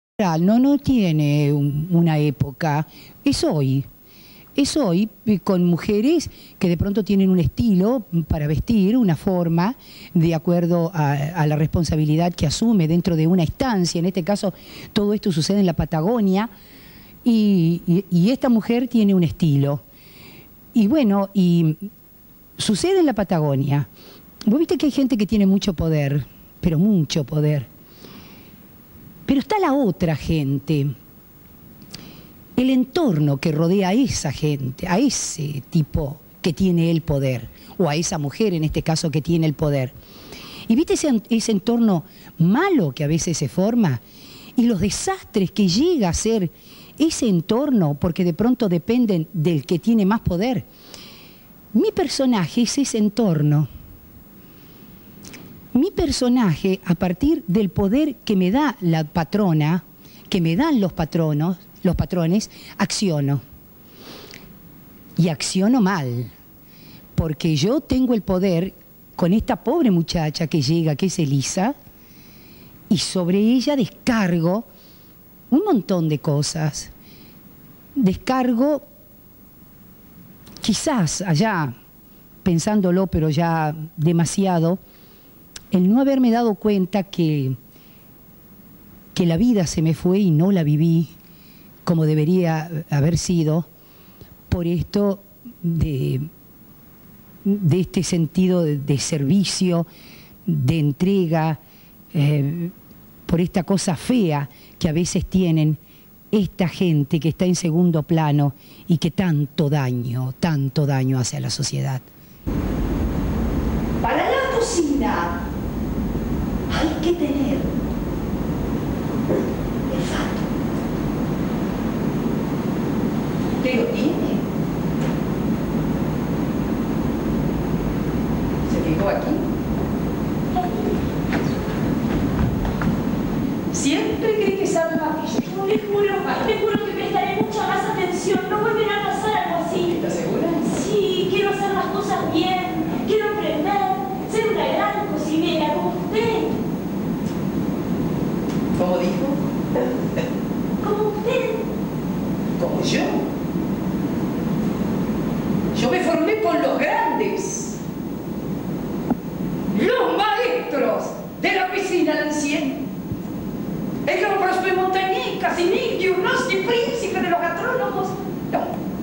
Norma Pons entrevistada por Matias Wullich. TEATRO.
La Actriz Norma Pons es entrevistada por Matias Wullich para el programa de tv. MWTV.
La Actriz protagoniza la Obra " cocinando con Elisa" en Teatro Del Pueblo, Buenos Aires . Argentina.